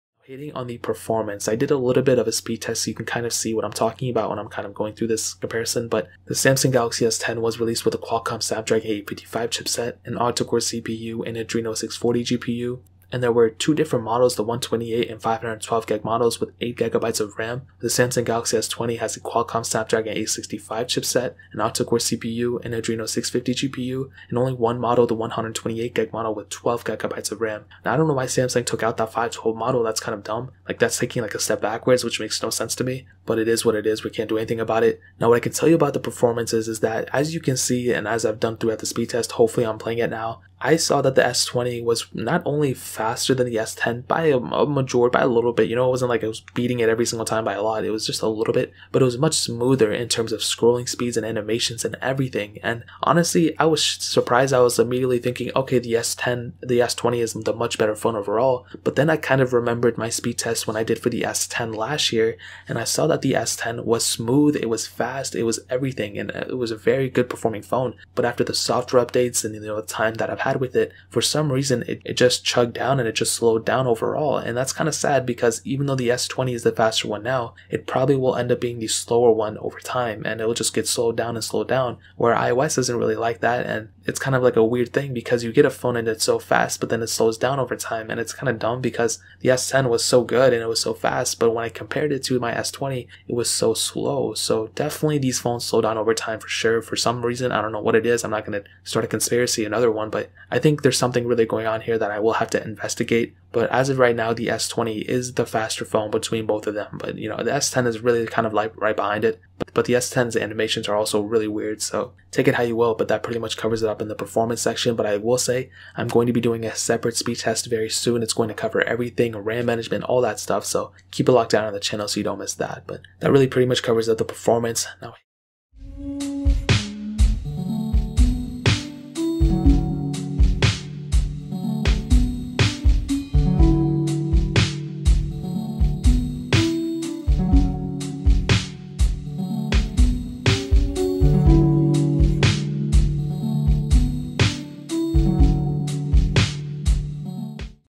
Samsung Galaxy S20 Vs Samsung Galaxy S10 Speed Comparison
Here is a quick comparison between the Samsung Galaxy S20 and the Samsung galaxy s10 speed comparison!